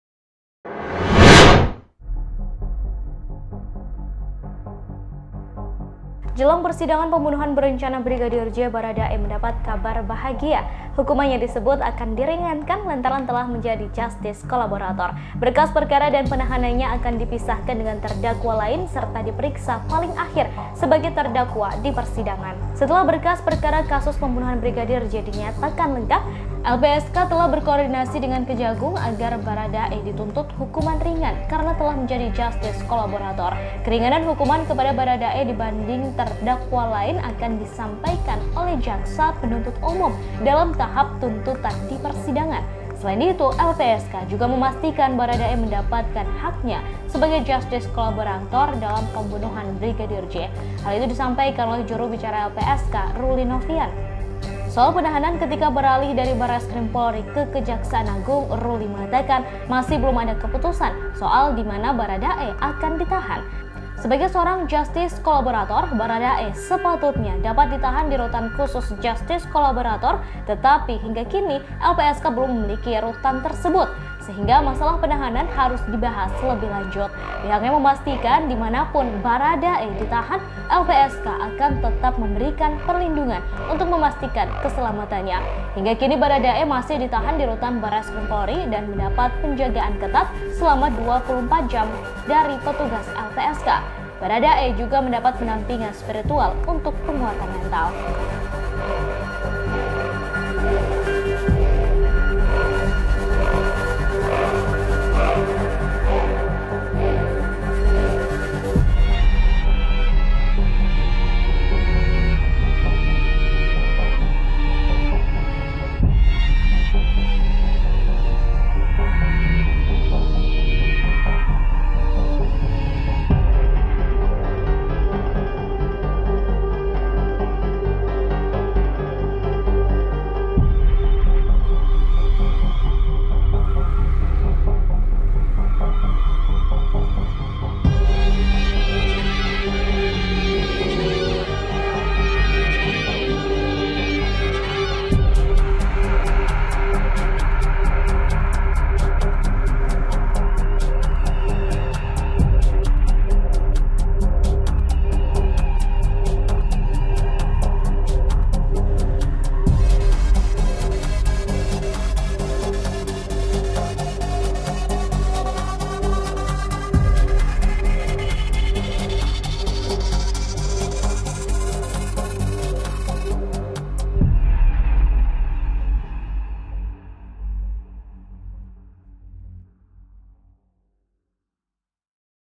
Bharada E Mendapat Kabar Bahagia Diberi Keringanan Hukuman karena Status Justice Collaborator
TRIBUNBANTEN - Jelang persidangan pembunuhan berencana Brigadir J, Bharada E mendapat kabar bahagia.

Hukumannya disebut akan diringankan lantaran telah menjadi Justice Collaborator.

Berkas perkara dan penahanannya akan dipisahkan dengan terdakwa lain serta diperiksa paling akhir sebagai terdakwa di persidangan.